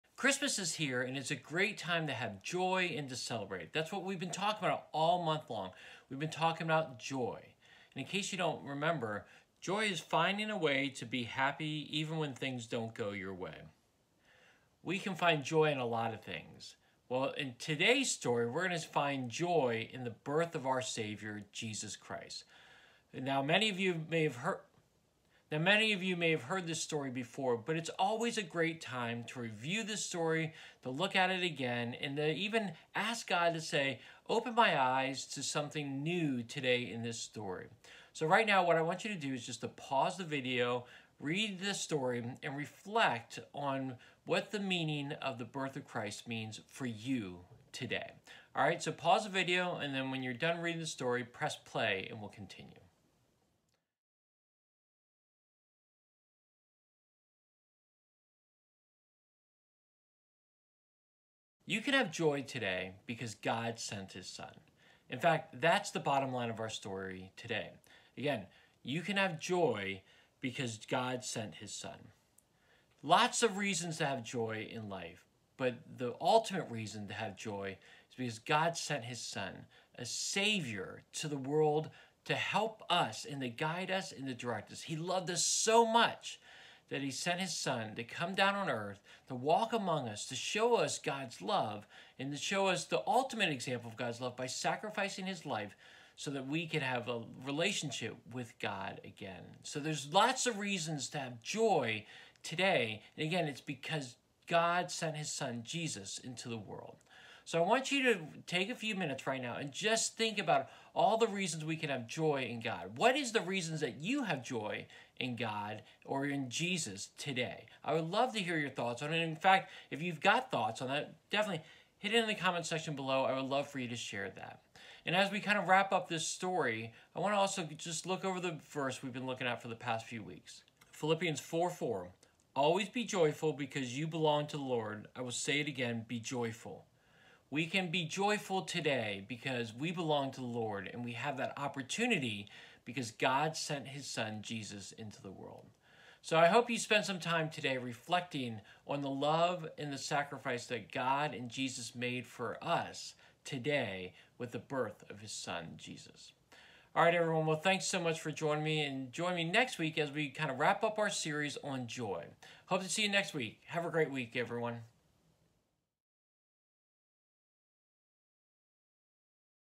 You can have Joy because God sent his son
Looking for Joy today? Discover why you can have JOY today because how he sent His son, Jesus for us.